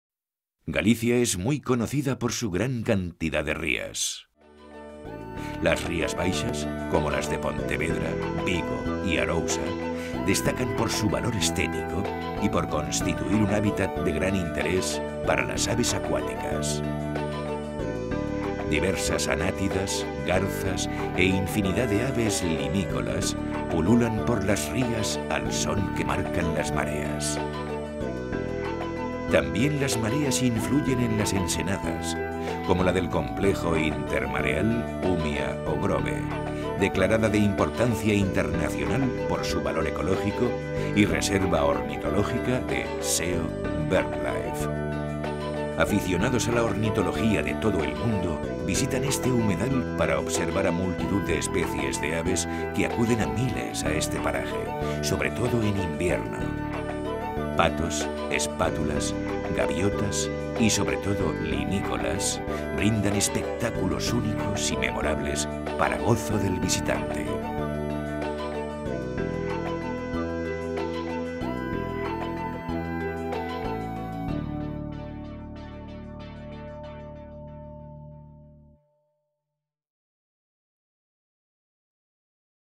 Turismo ornitológico en las rías de la provincia de Pontevedra
La provincia de Pontevedra cuenta con importantes emplazamientos en los que observar aves singulares durante las distintas estaciones del año.
El Complejo Intermareal Umia-O Grove (reserva ornitológica de SEO/BirdLife) es el mejor lugar de Galicia y uno de los mejores de España para la observación de aves acuáticas invernantes, tanto por la diversidad como por la sorprendente cantidad, ya que más de 18.000 pasan los meses fríos en este espacio, donde la espátula tiene la principal localidad de invernada y de paso migratorio.